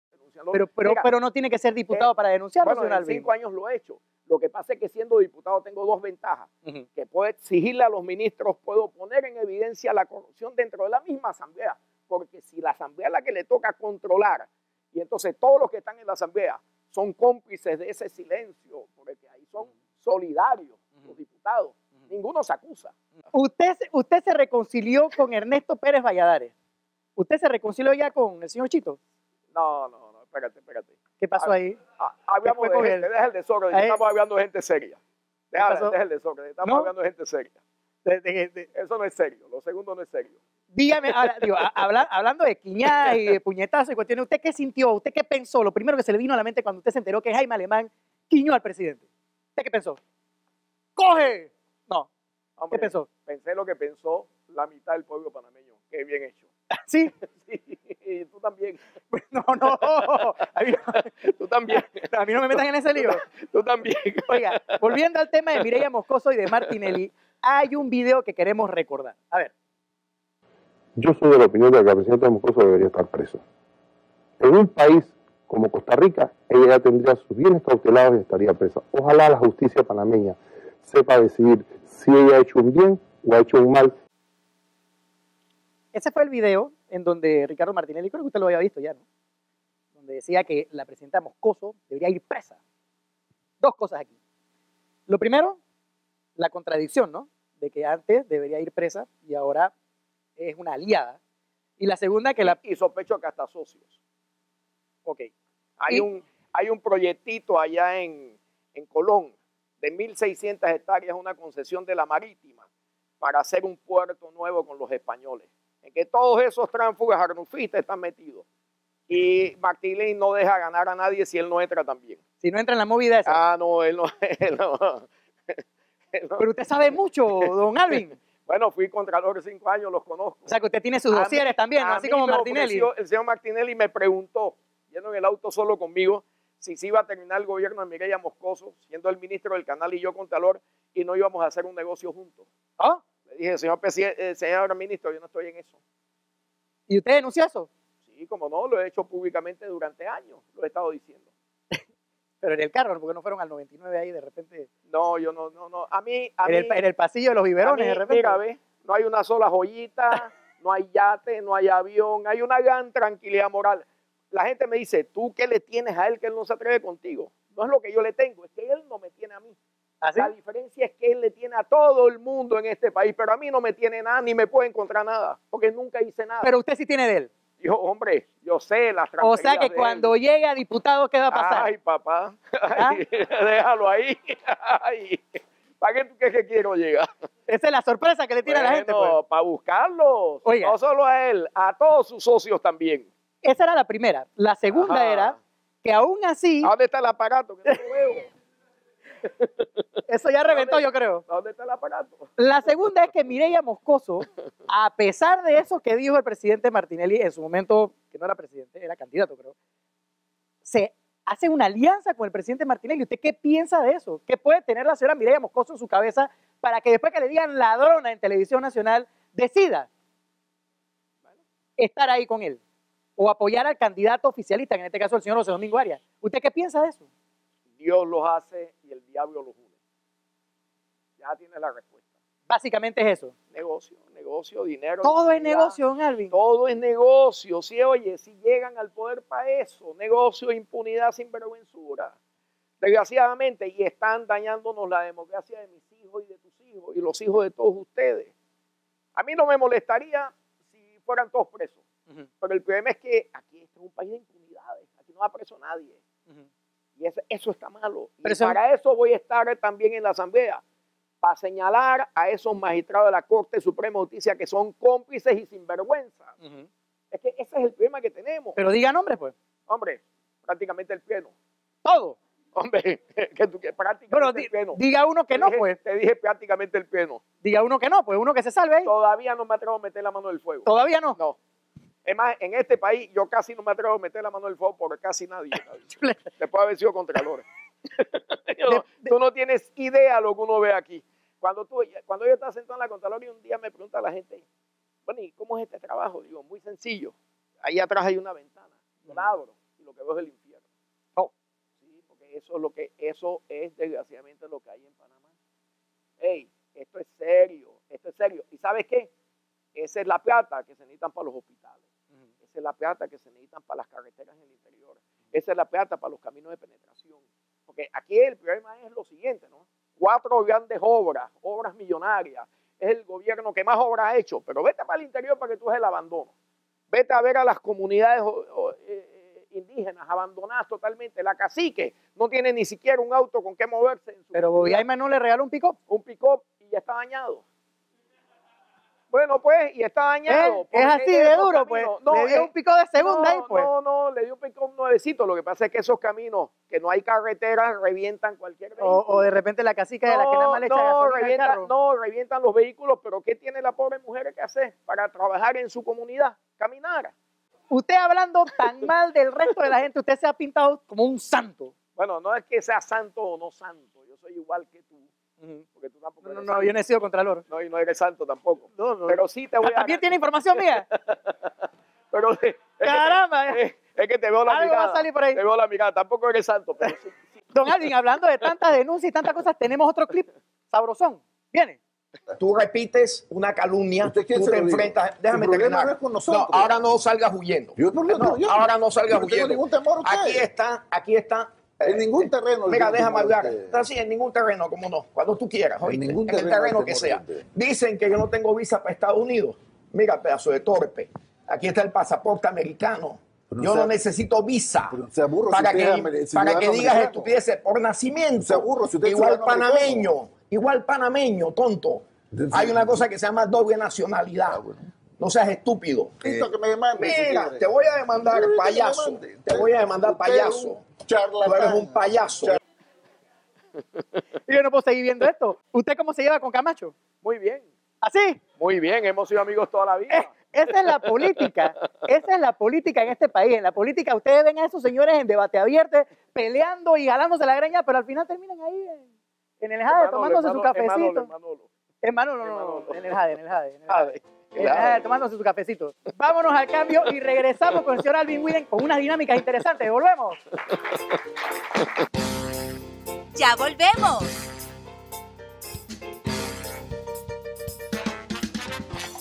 ENTREVISTA A ALVIN WEEDEN - PARTE 2
Segunda parte de la entrevista realizada por Eric Martínez (@emartineztv) al ex contralor y candidato independiente a diputado Alvin Weeden.